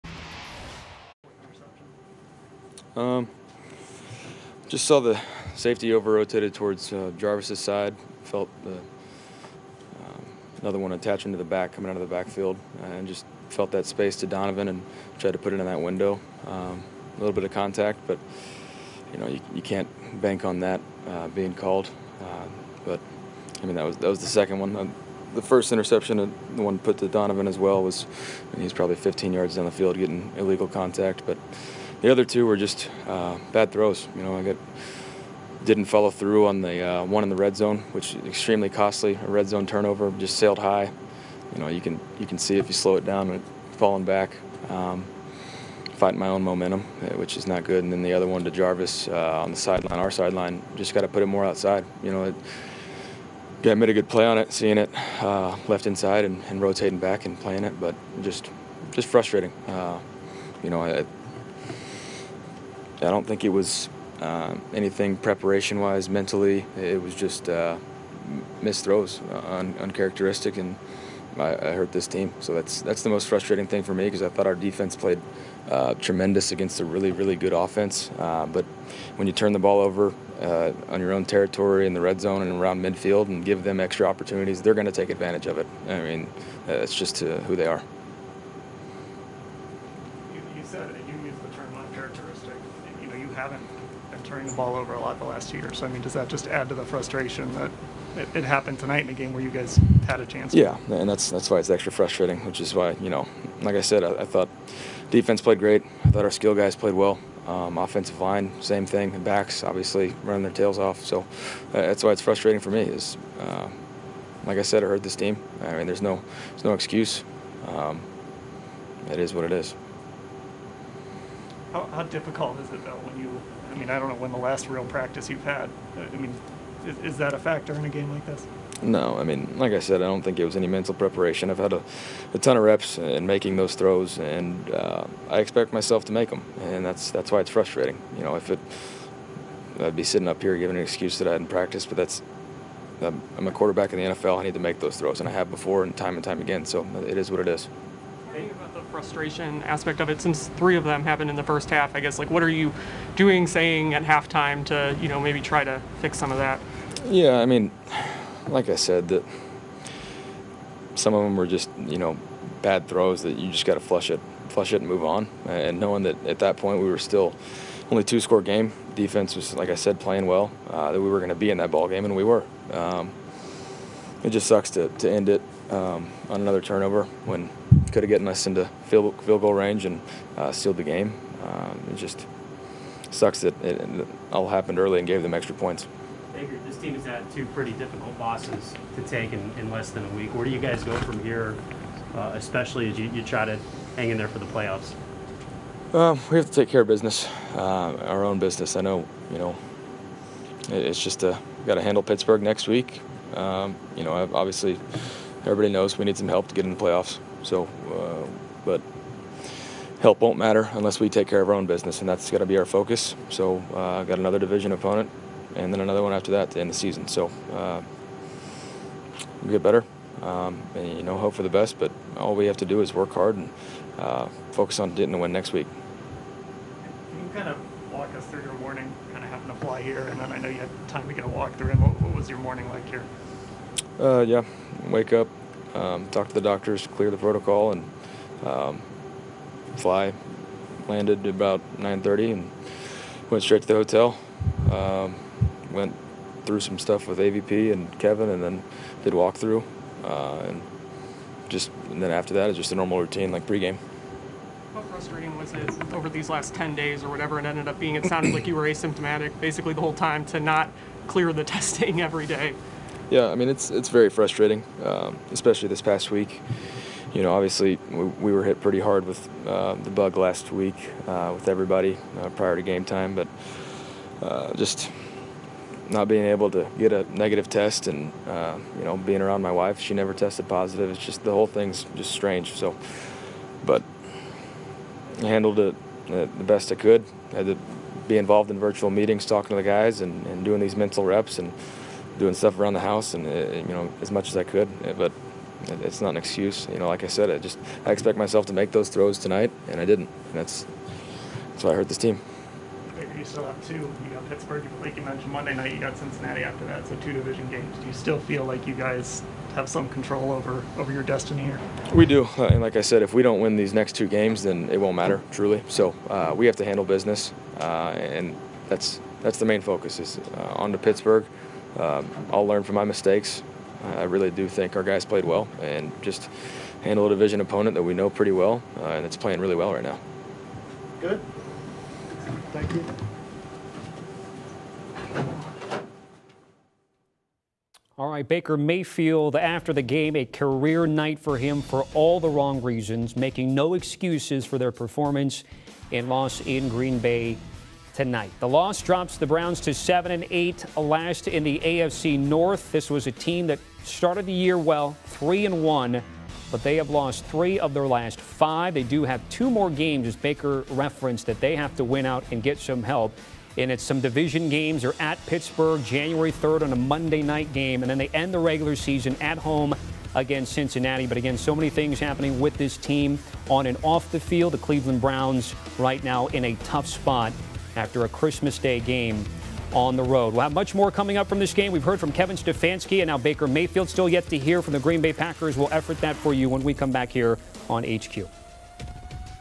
Baker Mayfield on Throwing 4 Interceptions in Loss vs Packers | CBS Sports HQ
Baker Mayfield speaks to the media following the Browns loss to the Packers.